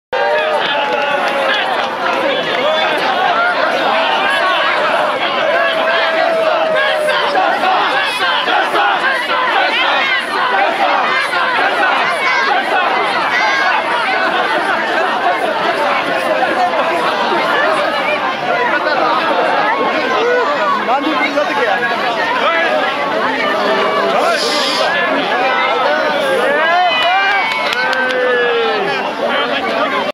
2018刈和野の大綱引き 勝利の歓喜に沸く上町
2018年2月10日  秋田県大仙市刈和野にて